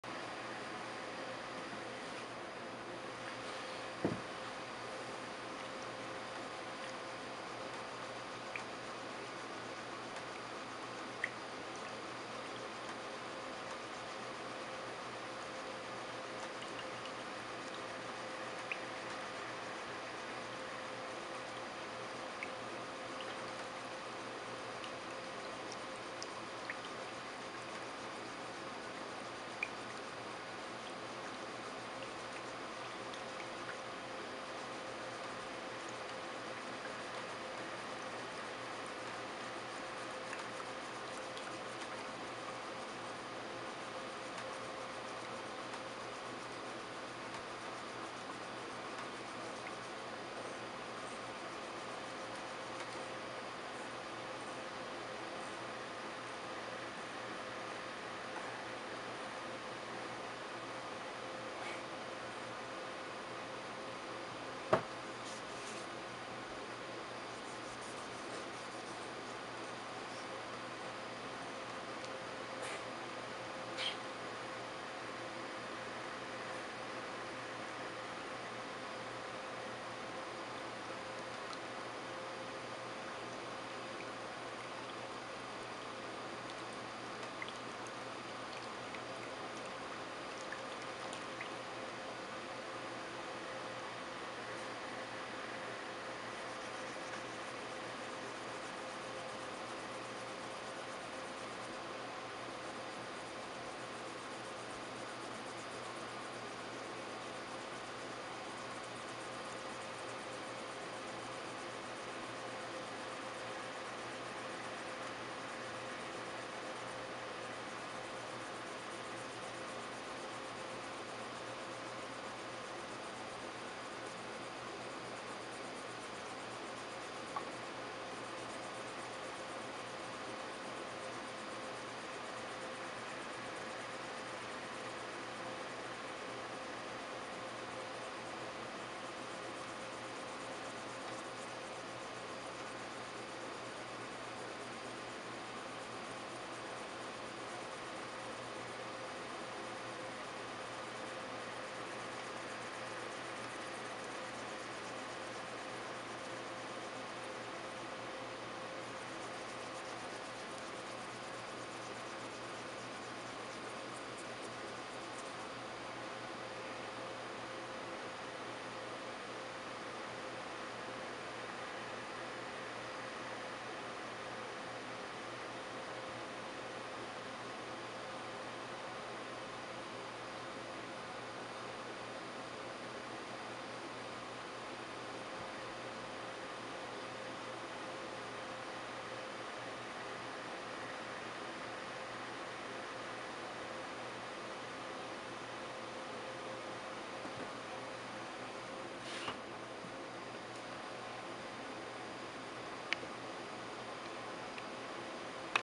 How to polish any gemstone with only sand paper.
How to polish a gemstone with only sand paper. Ok you need water also.